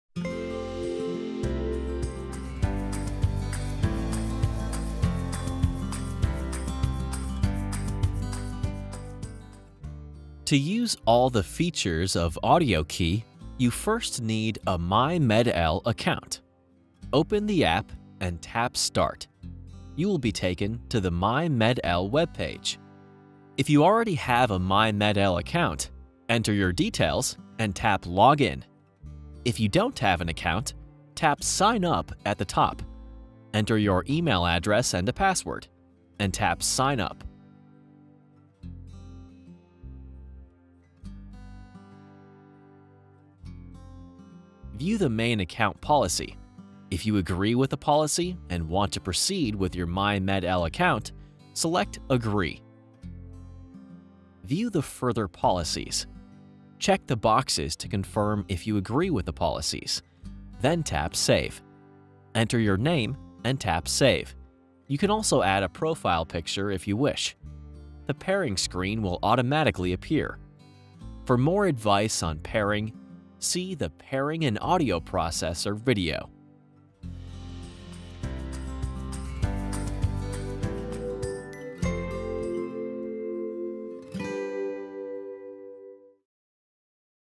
AudioKey Hands-On: Creating an Account | MED-EL
See how to set-up an account on the AudioKey app. With the AudioKey app, you can change settings, locate your audio processor, and check your hearing stats—all directly from your phone. This hands-on video is especially helpful for new cochlear implant recipients or for anyone involved with handling AudioKey such as parents, teachers or professionals.